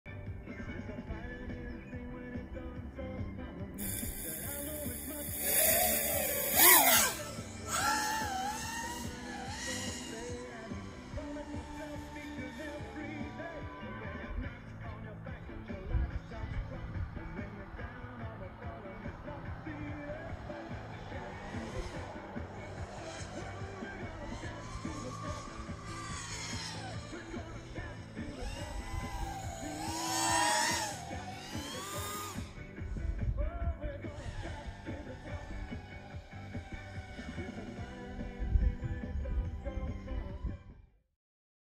Telegraph Point (Mid North Coast NSW) Boat ramp. 40 second rip! Environmental audio.
More ripping... More sound and video testing.